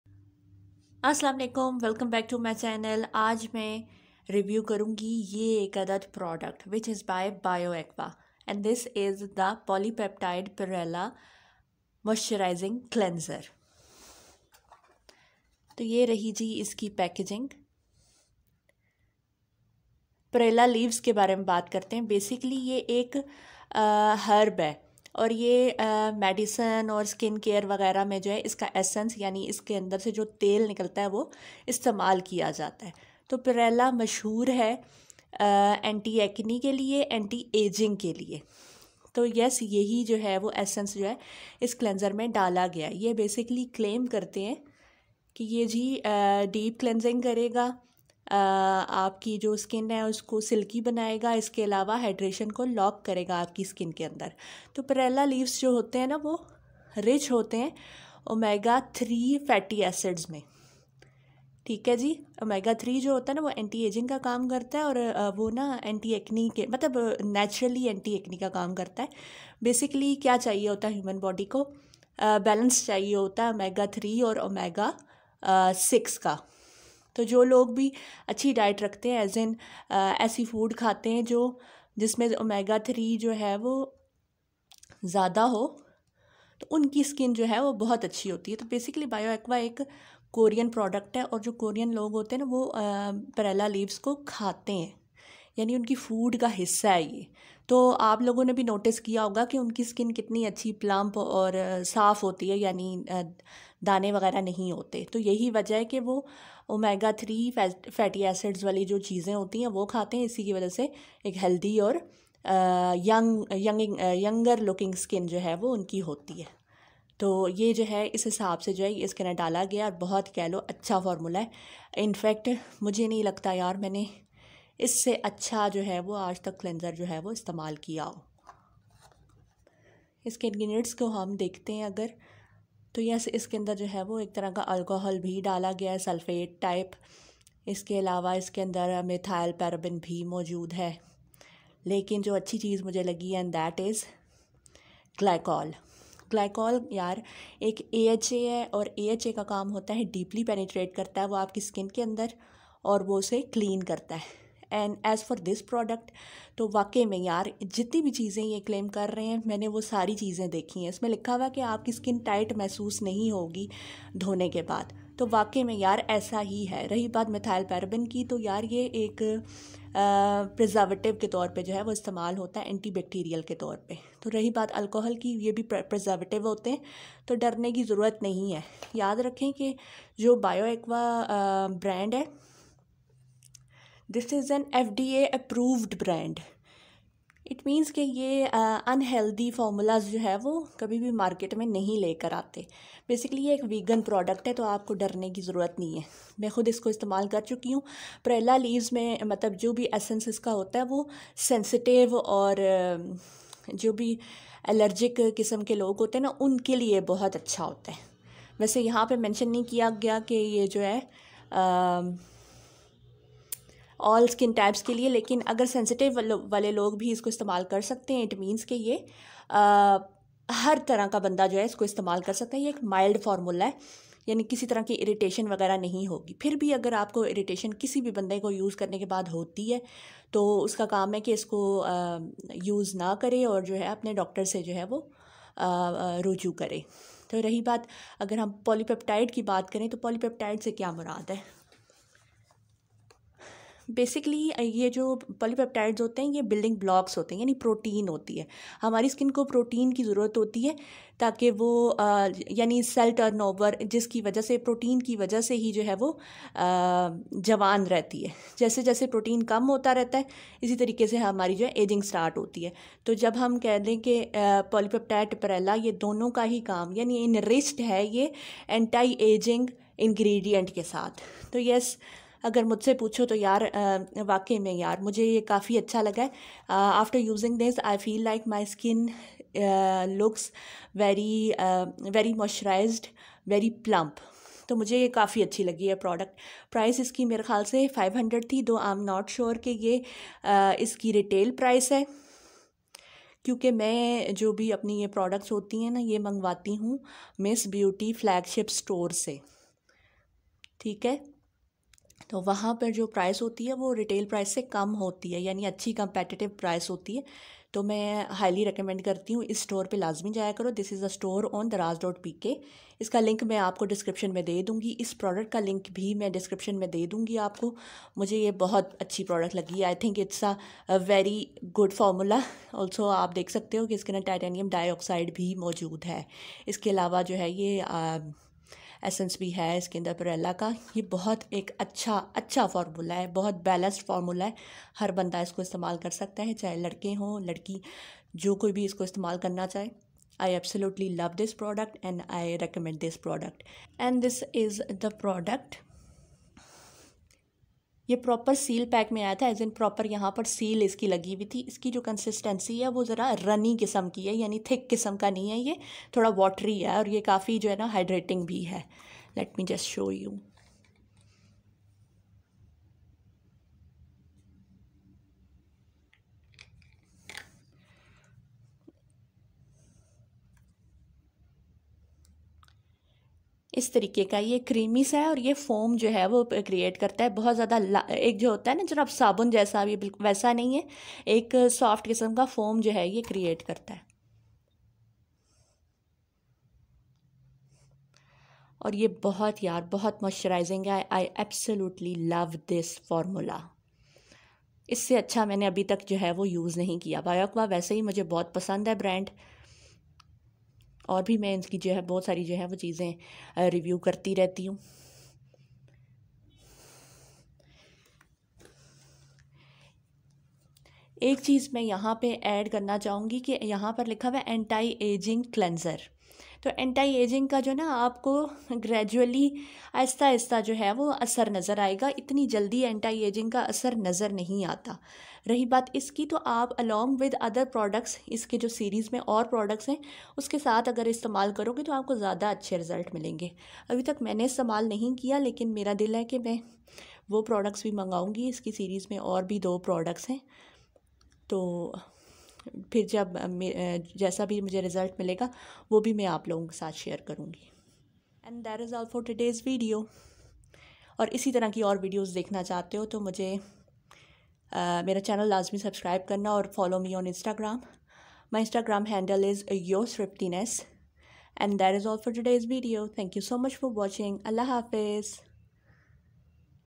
Bioaqua Polypeptide Perilla Cleanser Review | Miss Beauty Daraz.pk | Ara Malik
Hi, this is Ara Malik. My channel is mostly based on Reviews and Product Suggestions.